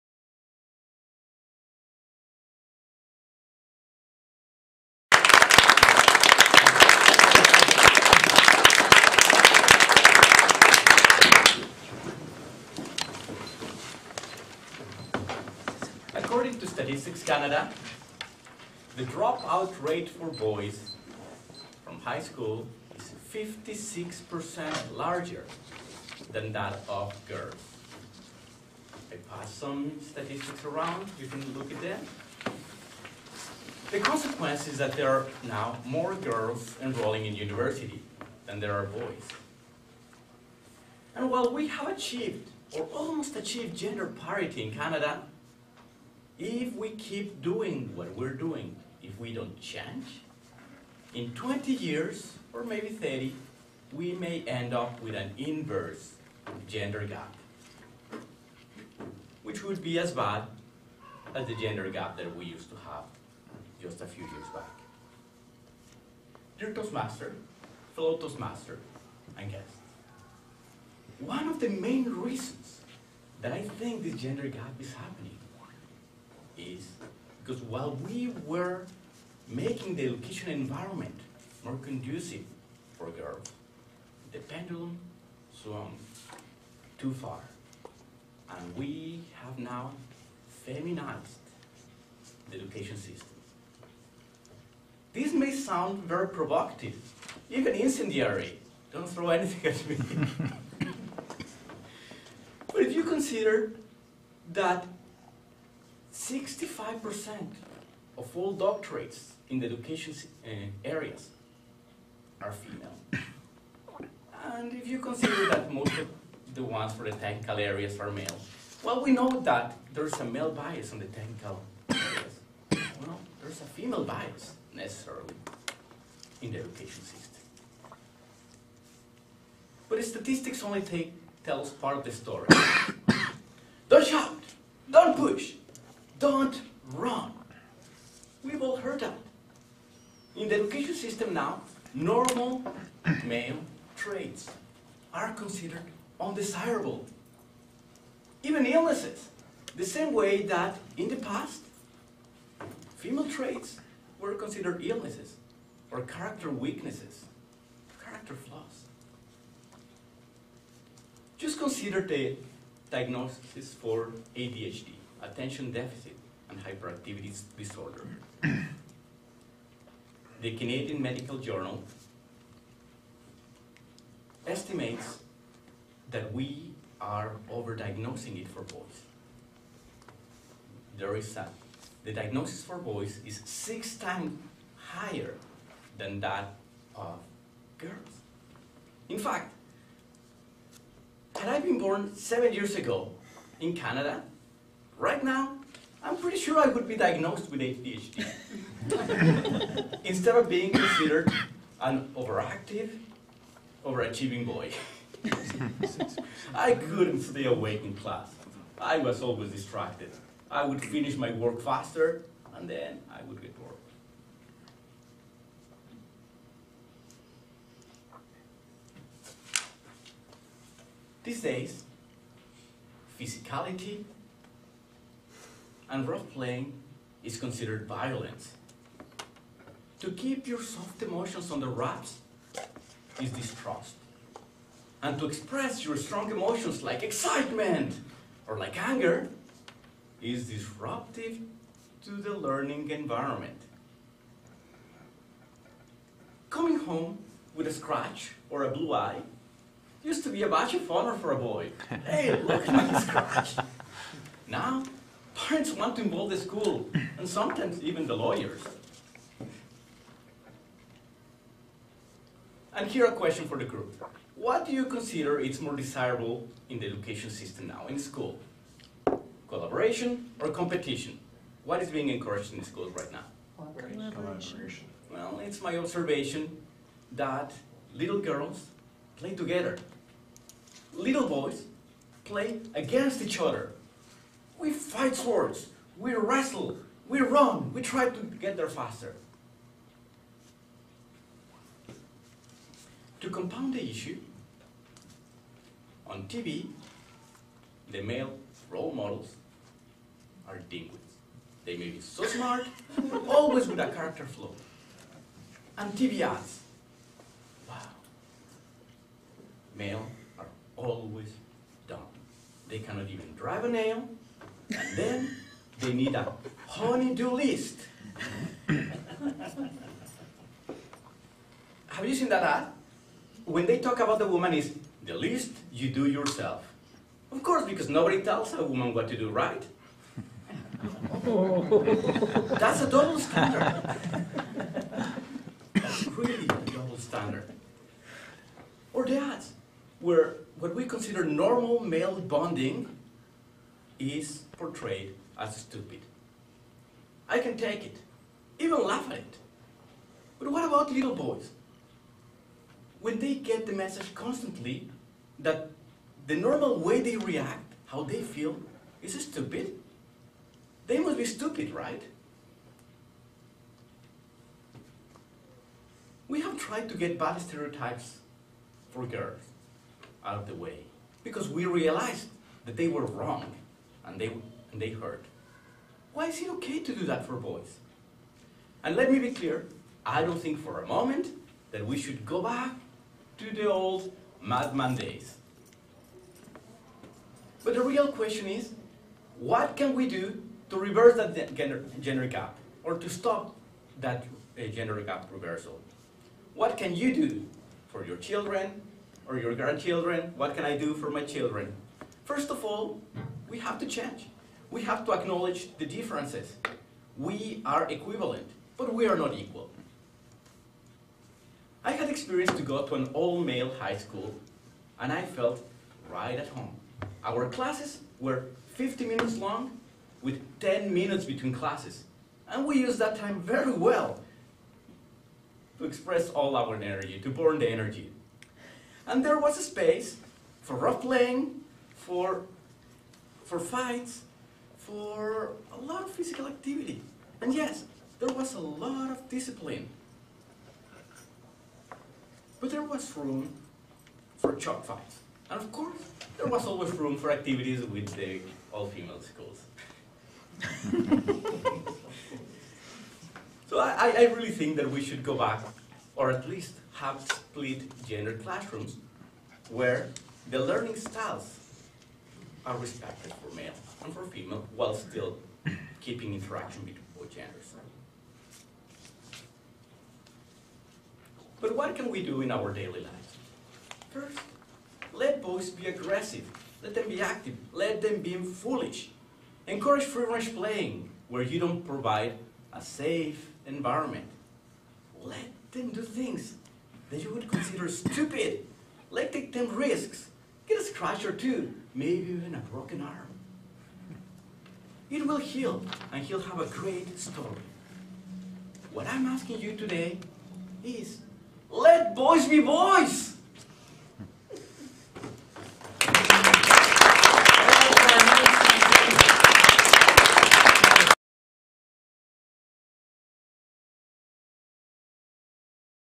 Toastmasters CC#9 Boys will be Boys
Critique of the educational environment where boys are not allowed to be boys.